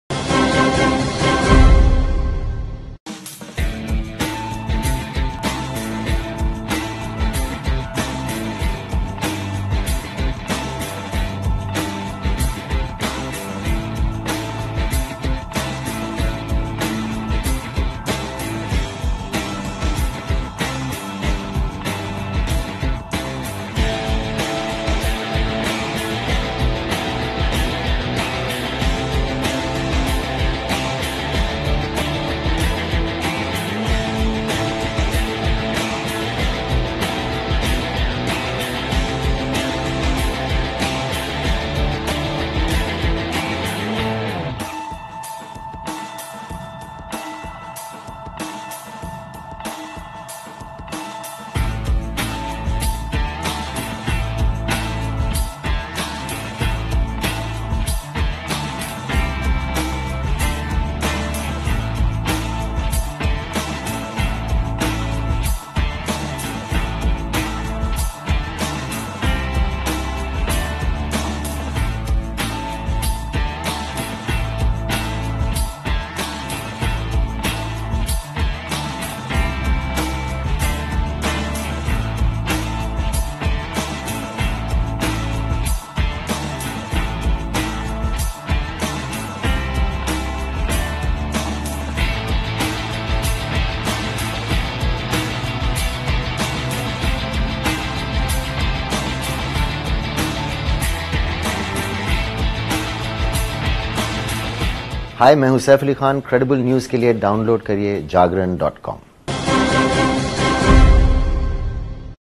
Neil Nitin Mukesh get blessing from Bollywood fraternity at his wedding reception!
Neil Nitin Mukesh and Rukmini Sahay hosted a lavish wedding reception where Bachchans, Rekha, Salman Khan, Katrina Kaif, Iulia Vantur, Bipasha Basu, Karan Singh Grover and many other Bollywoodsays showed their presence.Watch this scoop to know whose who attended the gala.